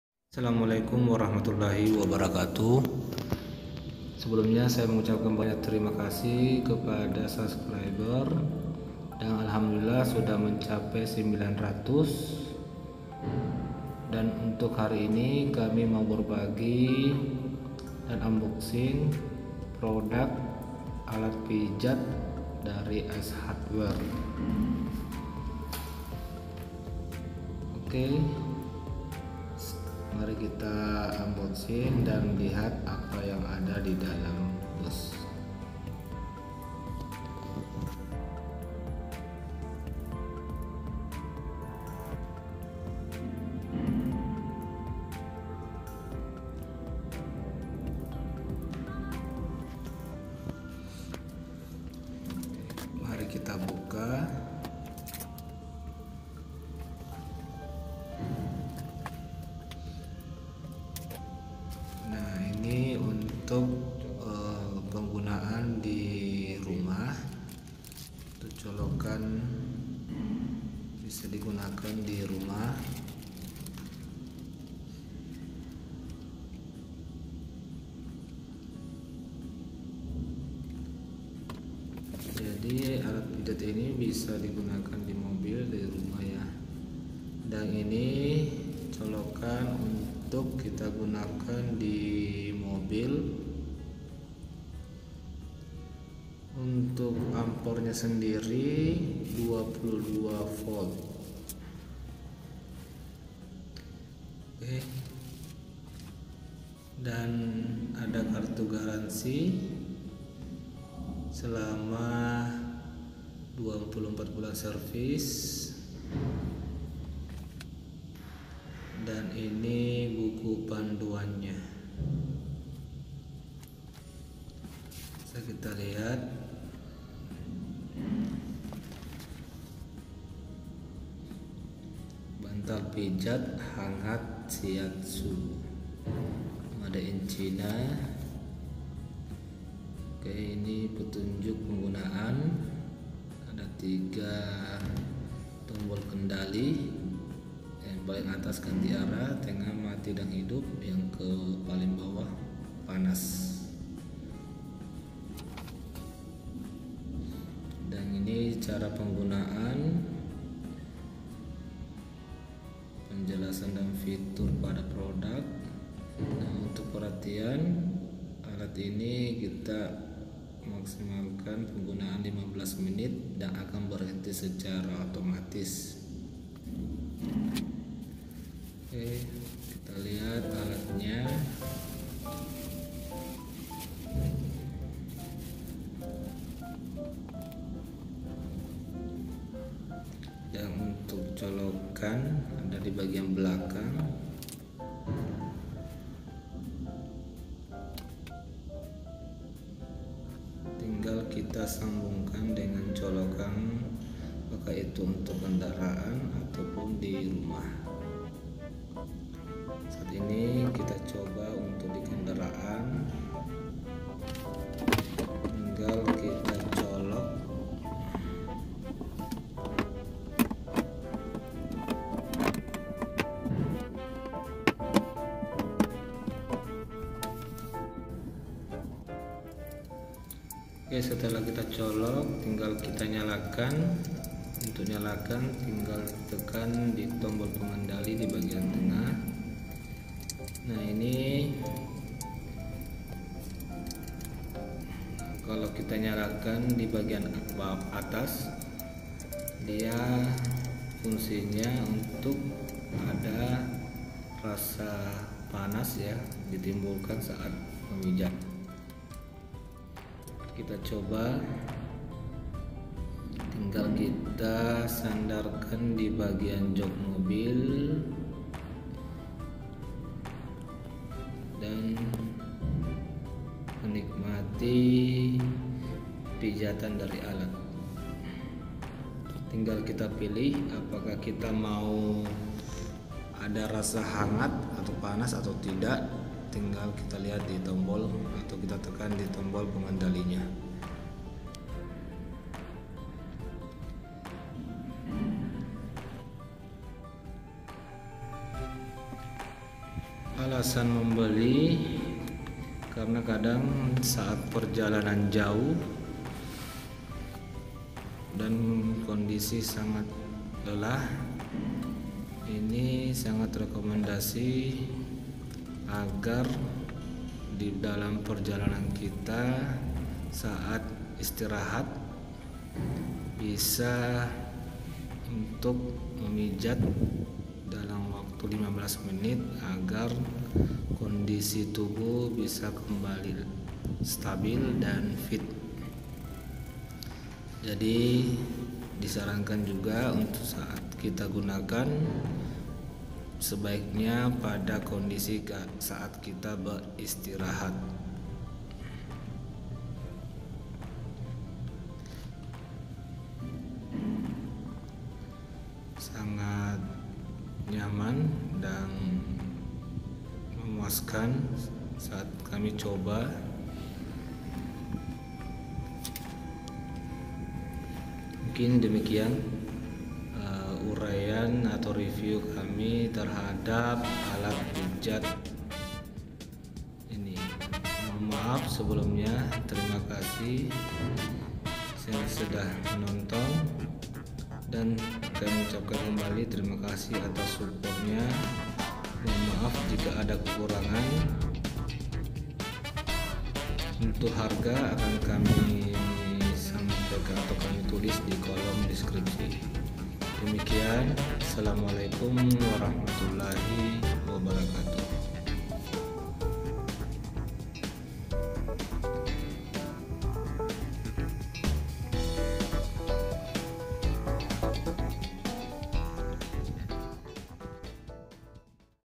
vlog 46 : Unboxing Alat Pijat Mobil
Untuk menjaga kondisi tubuh kita saat berkendara baik dalam perjalanan jauh maupun kemacetan, dengan alat ini bisa membantu mengembalikam kondisi tubuh menjadi lebih baik